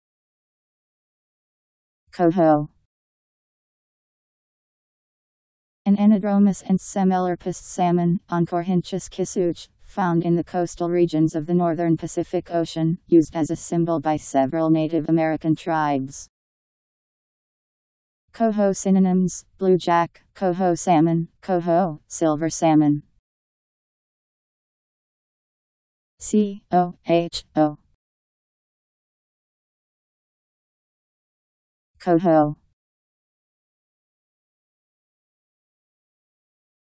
Coho Meaning
Video shows what coho means. An anadromus and semelparous salmon, Oncorhynchus kisutch, found in the coastal regions of the northern Pacific Ocean, used as a symbol by several Native American tribes.. coho synonyms: blue jack, coho salmon, cohoe, silver salmon.  Coho Meaning. How to pronounce, definition audio dictionary. How to say coho. Powered by MaryTTS, Wiktionary